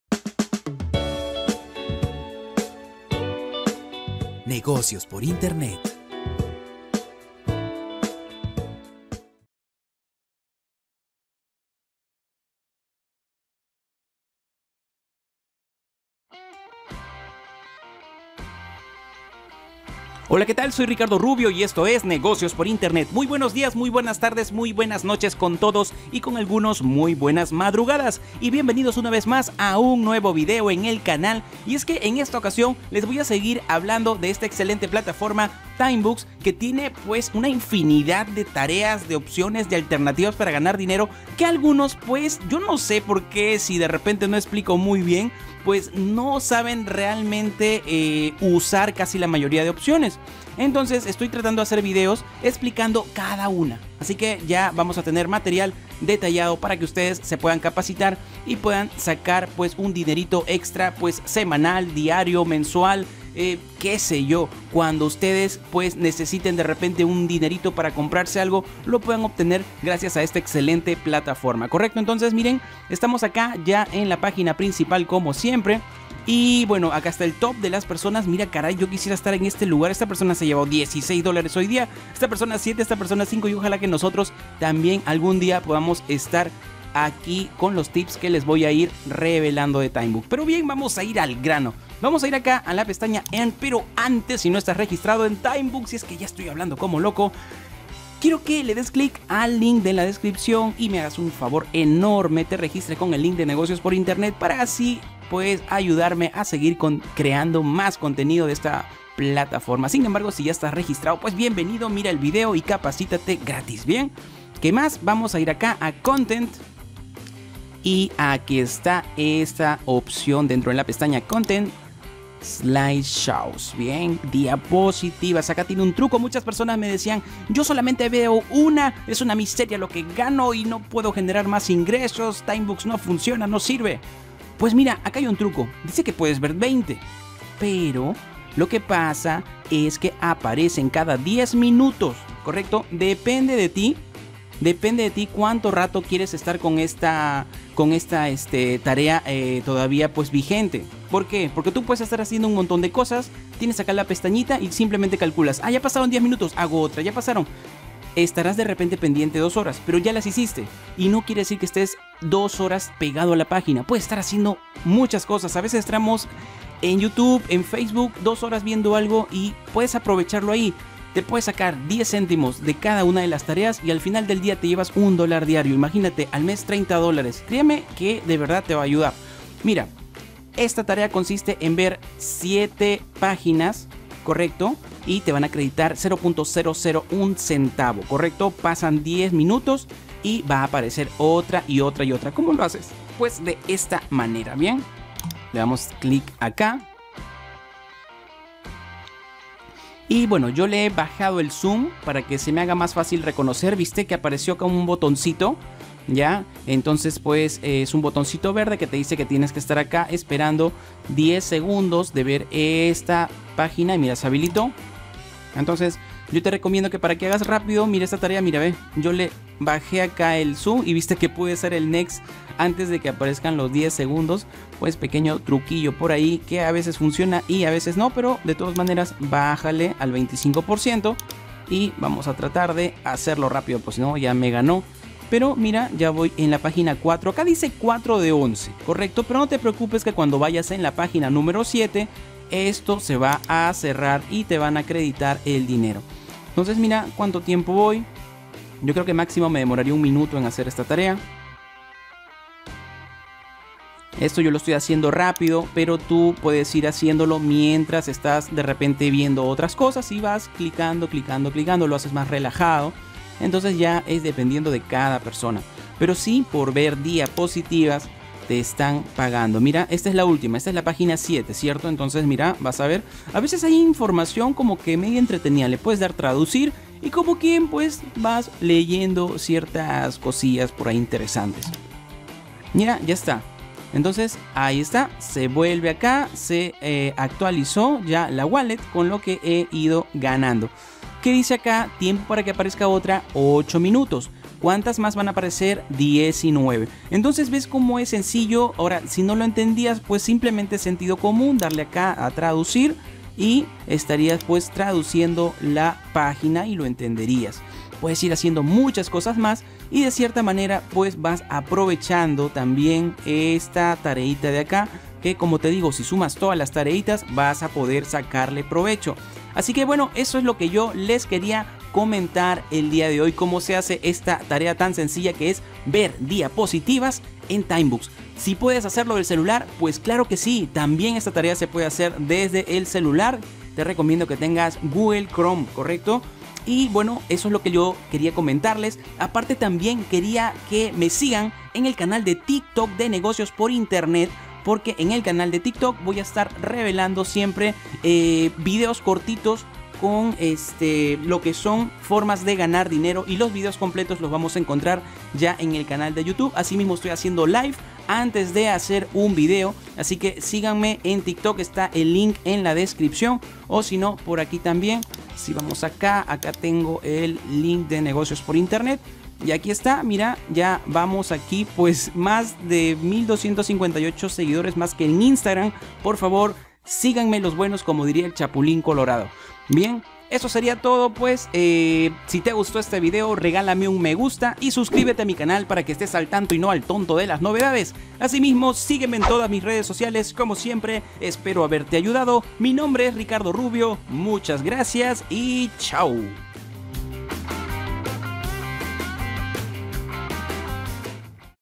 🔥 Como GANAR DINERO 💰 con TIMEBUCKS 📢 viendo DIAPOSITIVAS 💡 TIMEBUCKS SLIDESHOWS
Gana dinero con @TimeBucks  viendo DIAPOSITIVAS paso a paso

📌 LINK PARA REGISTARTE EN HIDEOUTTV Y RECIBIR 1 PUNTO DE MI PARTE
(Luego inicia sesion en ThePanda y PopJoy con tu mismo usuario de HideoutTV)